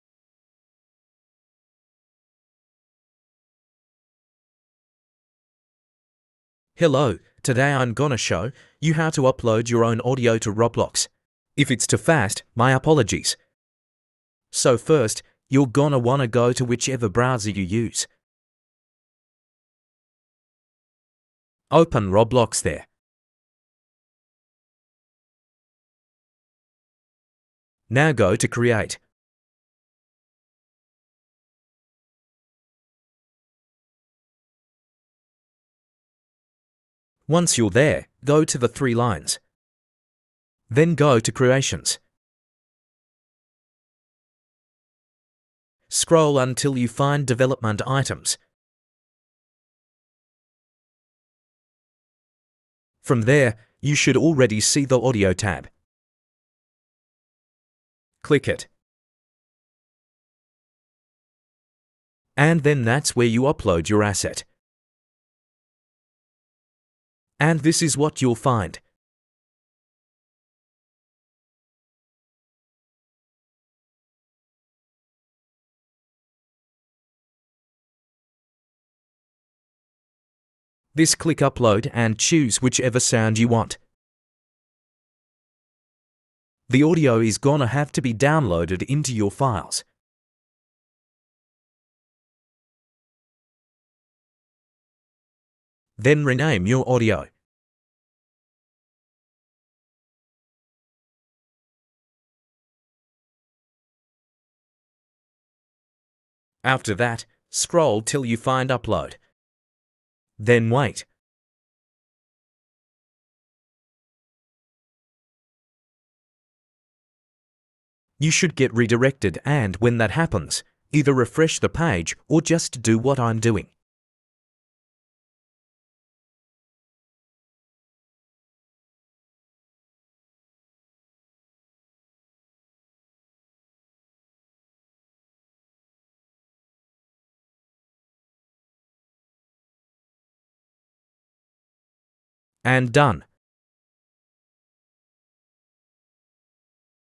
How to upload your own audio to Roblox.
I'm not the owner of the audio file you see in the video, I'm not trying to steal it. Check them out they're amazing at making these.